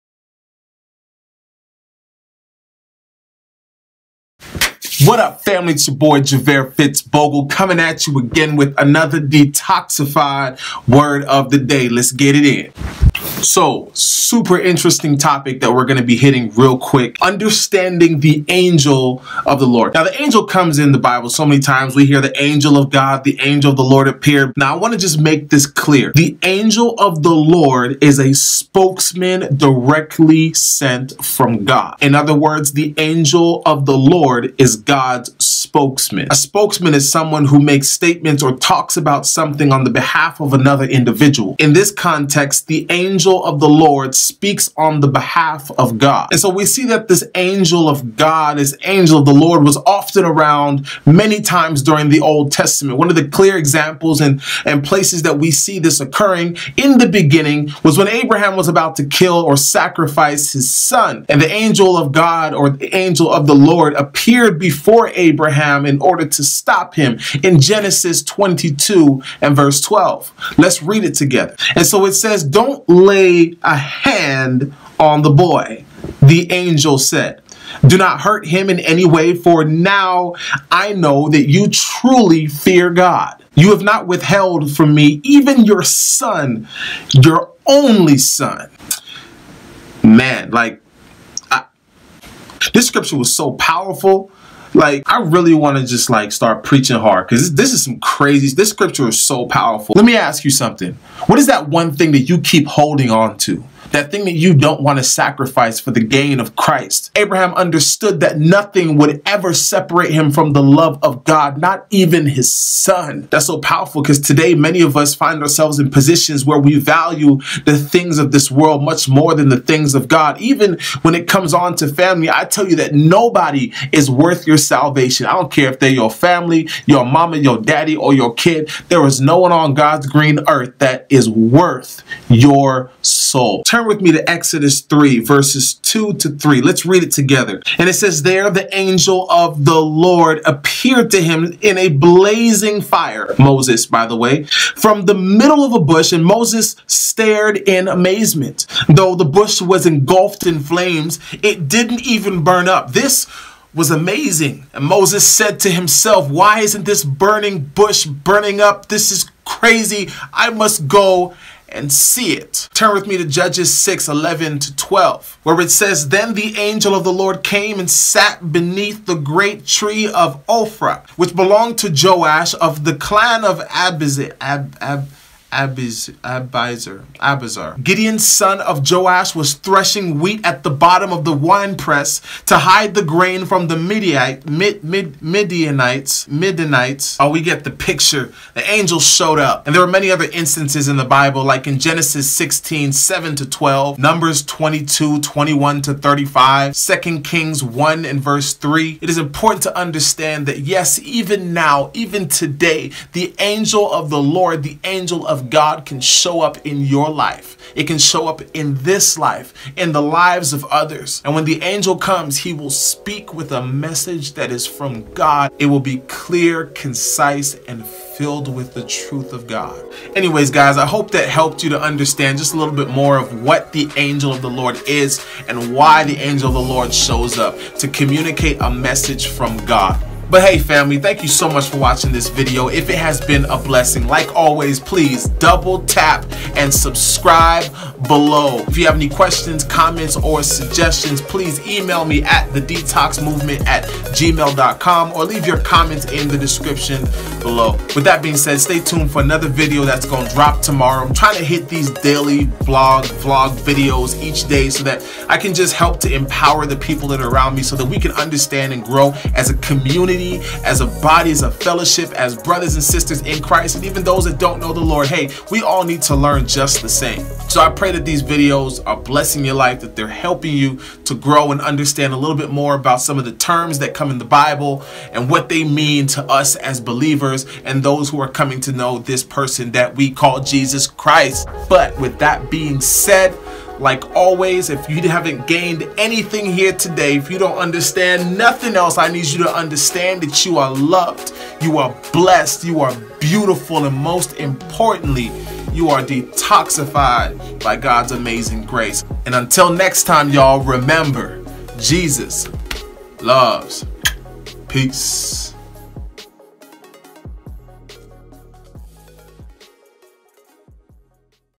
Who is the Angel of the Lord? // DEVO 002
Who is the Angel of the Lord? watch, share and comment.
(New DeTox Webisodes Every WEDNESDAY & FRIDAY)

MUST WATCH In today's Daily DeTox Devotional we will discuss briefly the question, "What is the Angel of the Lord?" We hear all these talks and thoughts of the Angel of the Lord, not to be confused with the spirit of God. Check out what I've got to say (@JavaireFitzBogle ).